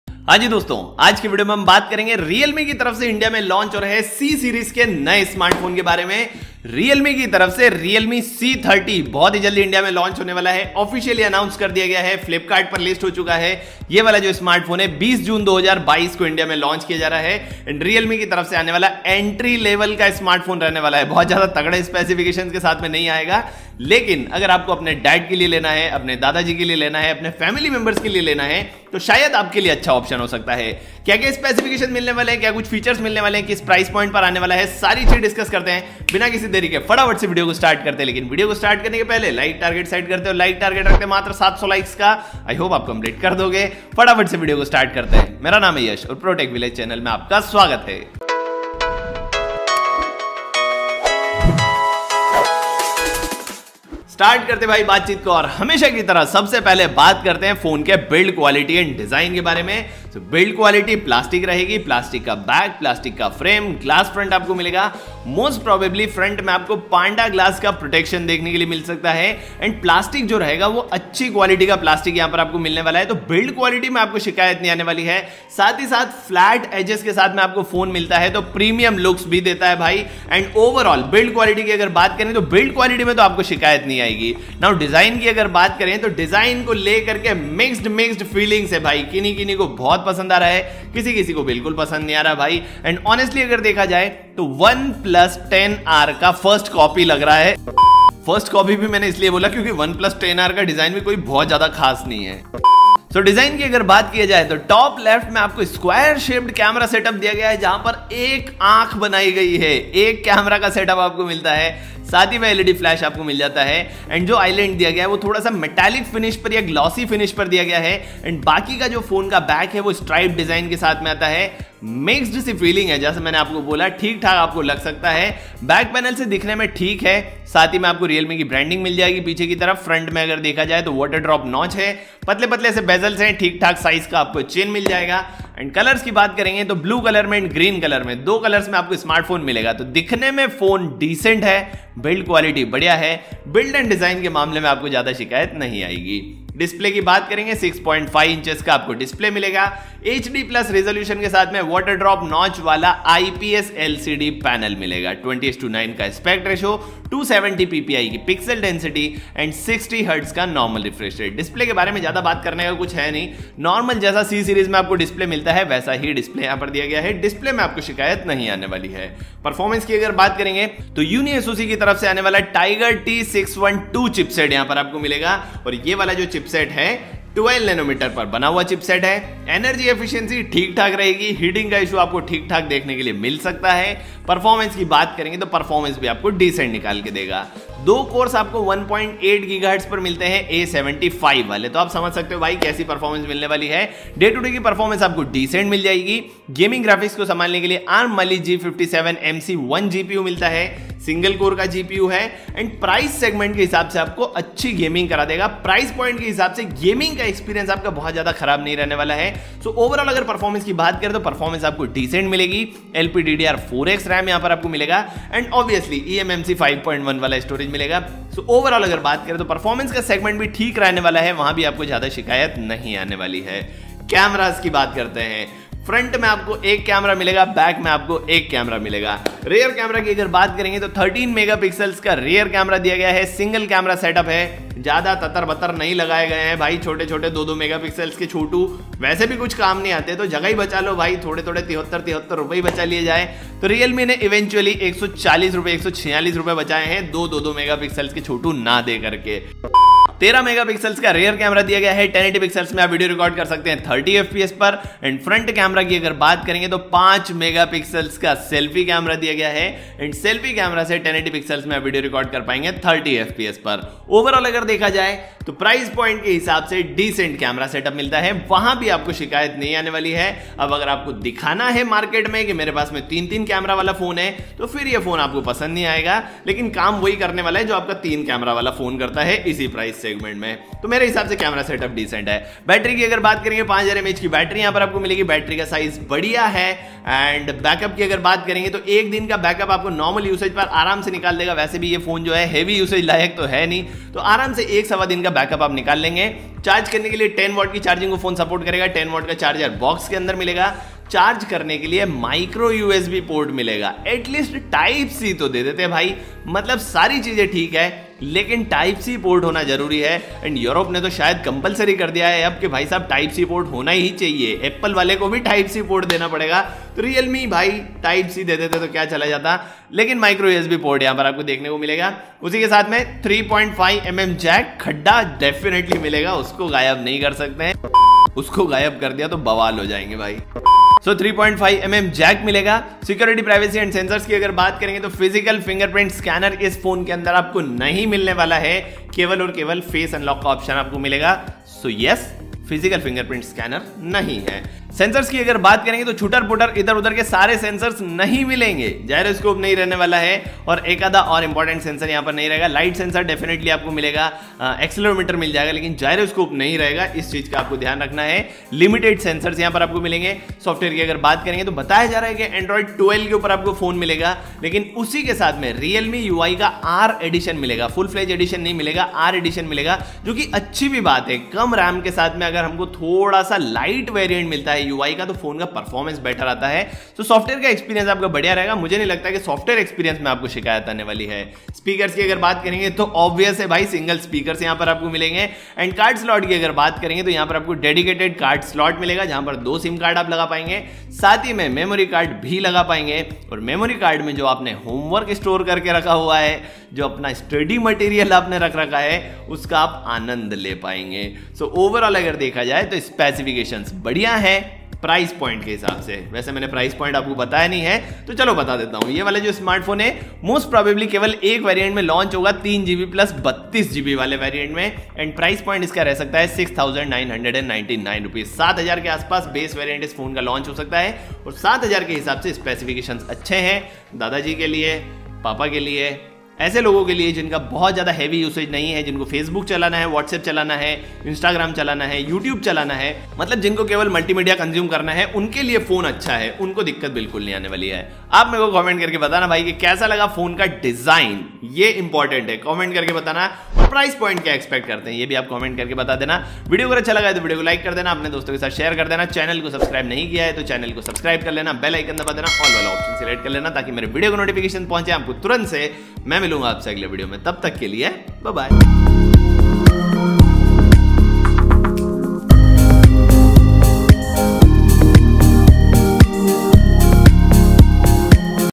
REALME C30 🔥🔥 - Best Phone for DADAJI under Rs.7000..? [HINDI]
Hello Guys,

About this video:

Hello Guys,

Is video me humne baat ki hai REALME C30 k specifications and features ki. I hope apko ye video pasand aayega.

Thanks